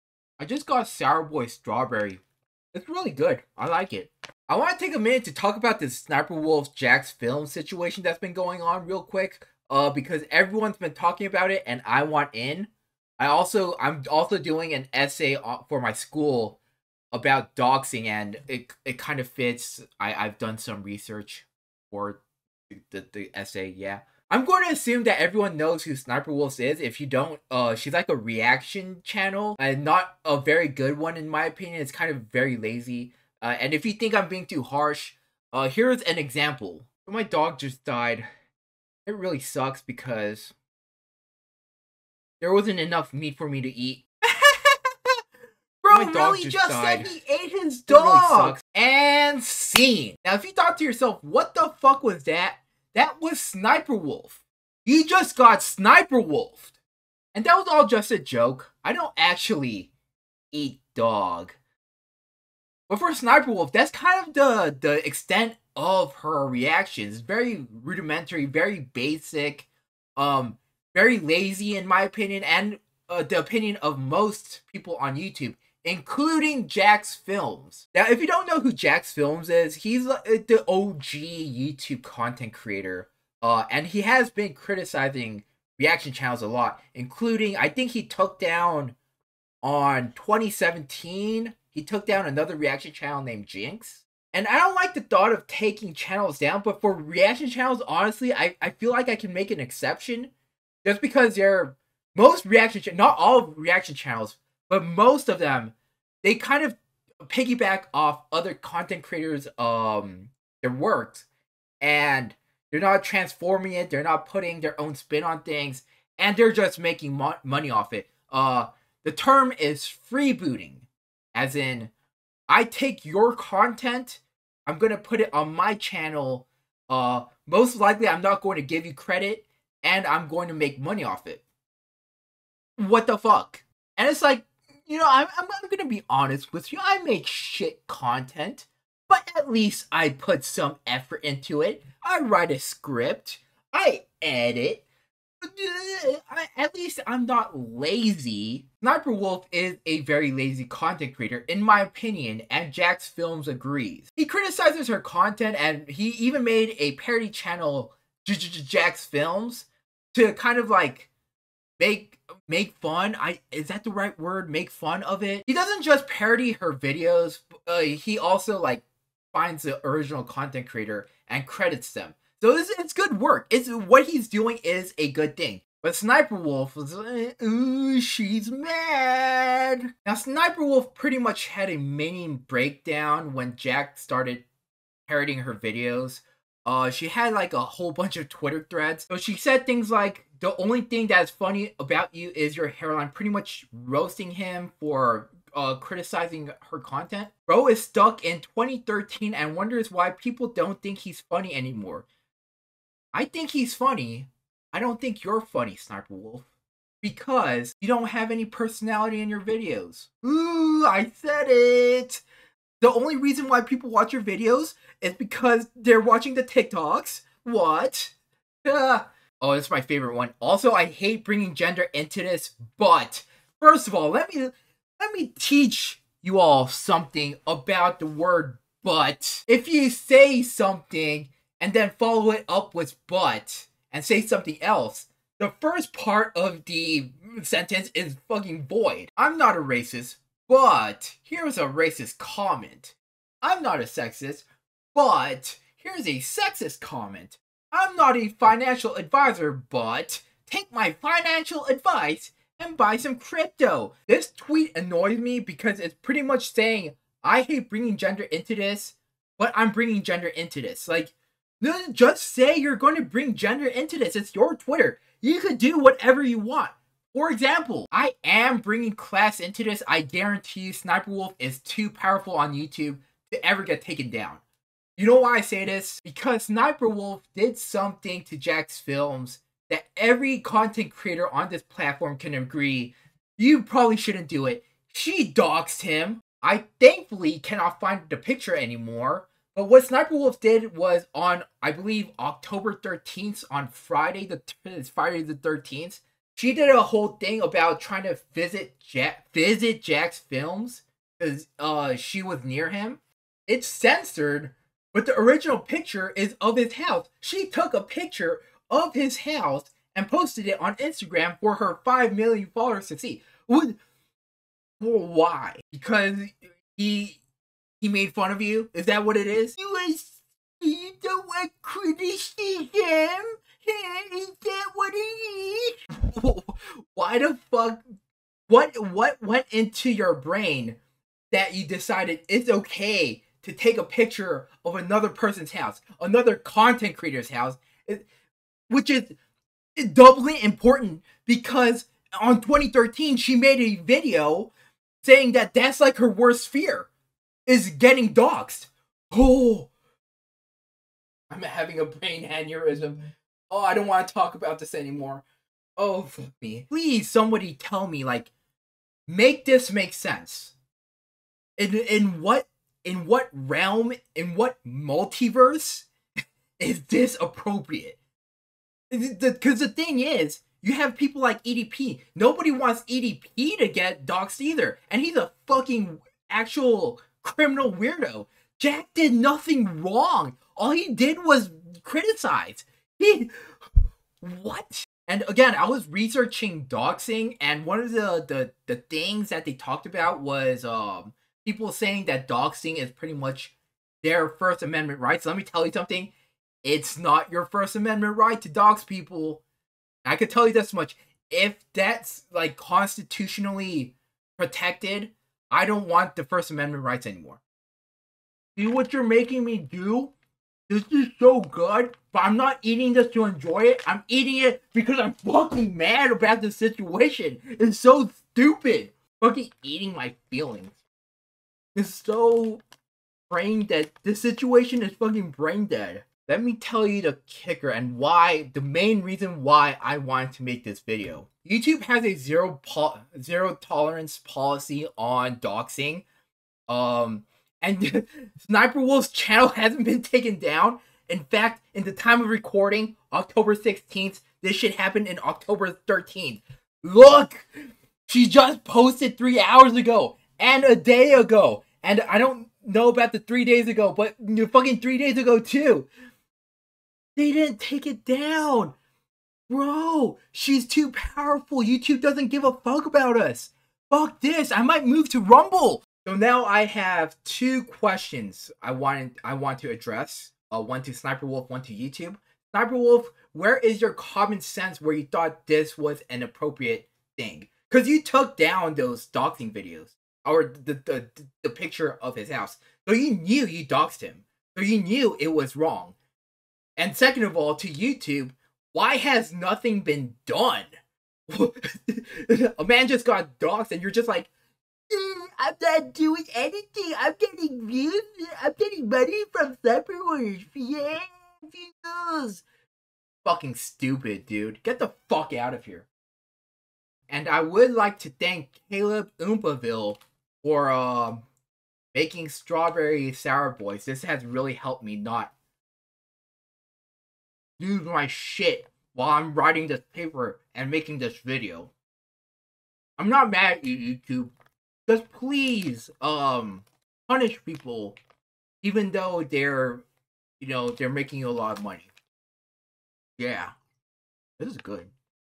SSSniperwolf is Unsurprisingly Dumb
What common sense is there that makes what SSsniperwolf remotely okay? Also based on the past couple days, it seems like she's going to get away with it.